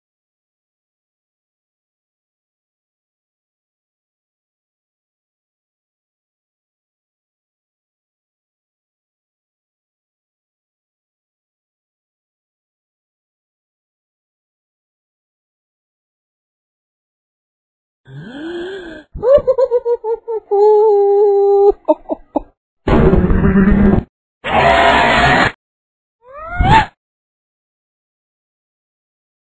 Gross runaway brain cartoon
Dude is in an insane asylum, in a padded cell, and his brain escapes through the clever use of a... chain saw?? Animated by Don Mangan and Artie Romero, using Adobe Animate cc software. See more of their work at:

ARG! Cartoon Animation LLC
Stilwell, Oklahoma USA